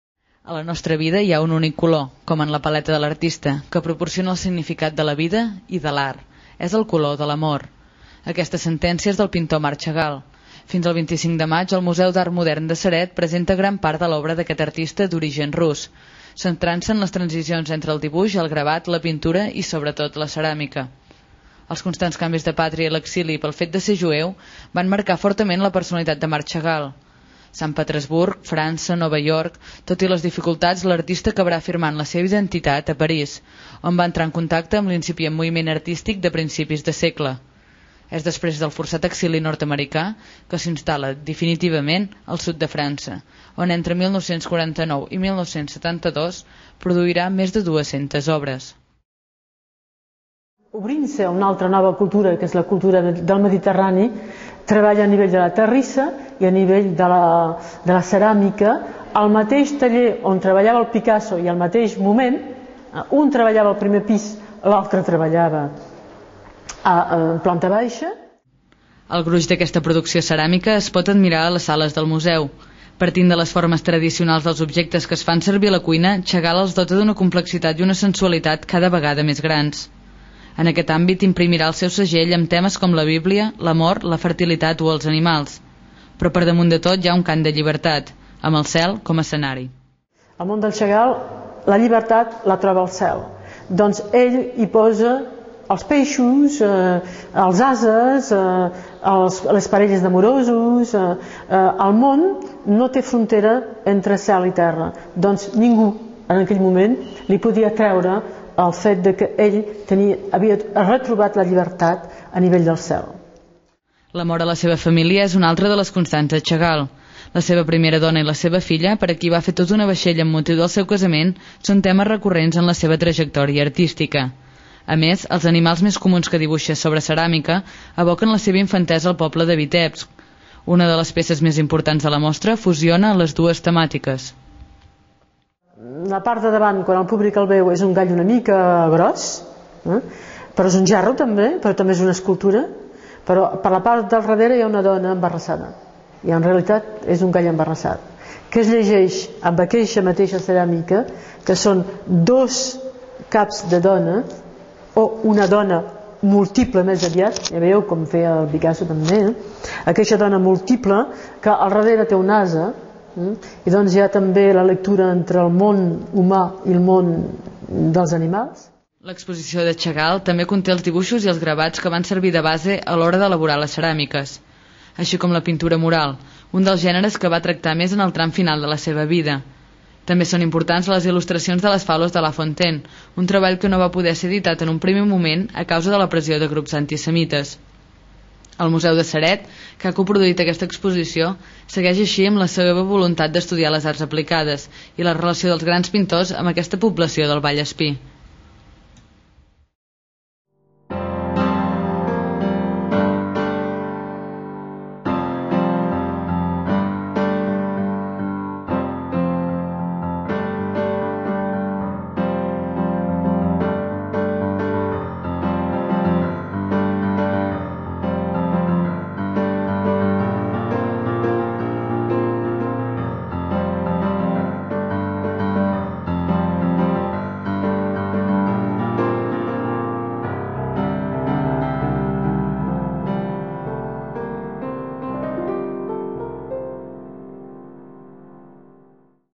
"Chagall troba la llibertat al cel"
La ceràmica de Marc Chagall (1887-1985), al Museu d'Art Modern de Ceret. L'exposició comprèn més de dues-centes obres de l'artista d'origen rus, que va viure molts anys al sud de França i que es va inspirar en el cel com a símbol de llibertat. La conservadora del Museu ens dóna algunes pistes per entendre la seva obra.